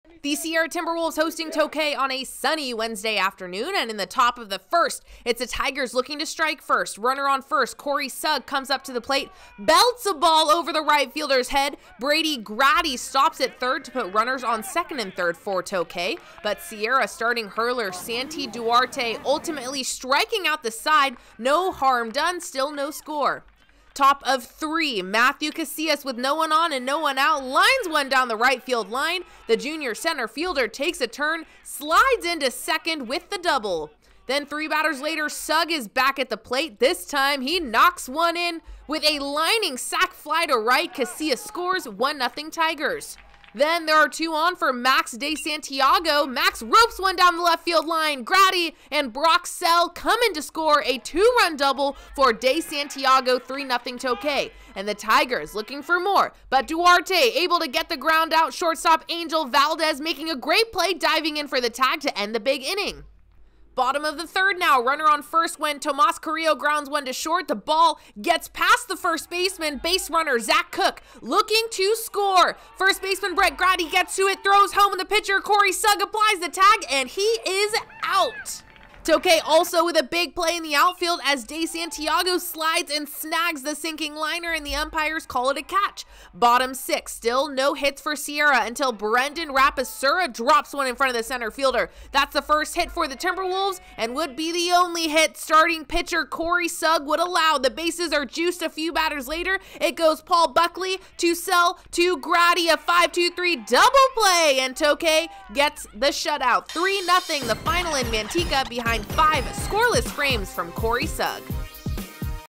Tokay at Sierra Baseball 3.2.22
THE SIERRA TIMBERWOLVES HOSTING TOKAY ON A SUNNY WEDNESDAY AFTERNOON.
AND IN THE TOP OF THE FIRST... IT'S THE TIGERS LOOKING TO STRIKE FIRST. RUNNER ON FIRST... CORY SUGG COMES UP TO THE PLATE AND BELTS A BALL OVER THE RIGHT FIELDERS HEAD. BRADY GRADDY STOPS AT THIRD TO PUT RUNNERS ON SECOND AND THIRD FOR TOKAY.
BUT.. SIERRA STARTING HURLER SANTI DUARTE WOULD ULTIMATELY STRIKE OUT THE SIDE. NO HARM DONE. STILL NO SCORE.
TOP OF THREE... MATHREW CASILLAS WITH NO ONE ON AND NO ONE OUT LINES ONE DOWN THE RIGHT FIELD LINE... THE JUNIOR CENTER FIELDER TAKES A TURN AND SLIDES INTO SECOND WITH THE DOUBLE.
THREE BATTERS LATER... SUGG IS BACK AT THE PLATE AND THIS TIME, HE KNOCKS ONE IN WITH A LINING SAC FLY TO RIGHT. CASILLAS SCORES. 1-NOTHING TIGERS.
THEN... THERE ARE TWO ON FOR MAX DE SANTIAGO... MAX ROPES ONE DOWN THE LEFT FIELD LINE... GRADDY AND BROCK SELL COME INTO SCORE. A TWO RUN DOUBLE FOR DE SANTIAGO. 3-NOTHING TOKAY.
AND THE TIGERS LOOKING FOR MORE... BUT DUARTE ABLE TO GET THE GROUND OUT. SHORTSTOP ANGEL VALDEZ MAKING A GREAT PLAY.. DIVING IN FOR THE TAG TO END THE BIG INNING.
BOTTOM OF THE THIRD NOW... RUNNER ON FIRST WHEN TOMAS CARILLO GROUNDS ONE TO THE SHORT STOP... BUT THE BALL GETS BY THE FIRST BASEMAN...  AND BASERUNNER ZAC COOK IS LOOKING TO SCORE... FIRST BASEMAN BRETT GRADDY GETS TO IT... THROWS HOME... THE PITCHER CORY SUGG APPLIES THE TAG AND HE IS OUT AT HOME.
TOKAY, ALSO WITH A BIG PLAY IN THE OUTFIELD AS DE SANTIAGO SLIDES AND SNAGS THE SINKING LINER AND THE UMPIRES CALL IT A CATCH.
BOTTOM SIX... STILL NO HITS FOR SIERRA UNTIL... BRENDAN RAPISURA DROPS ONE IN FRONT OF THE CENTER FIELDER. THATS THE FIRST HIT FOR THE TIMBERWOLVES AND WOULD BE THE ONLY HIT STARTING PITCHER CORY SUGG WOULD ALLOW.
THE BASES ARE JUICED A FEW BATTERS LATER... BUT IT GOES PAUL BUCKLEY... SELL... GRADDY. 5-2-3 DOUBLE PLAY AND TOKAY GETS THE SHUT OUT. 3-NOTHING THE FINAL IN MANTECA BEHIND FIVE SCORELESS FRAMES FROM CORY  SUGG.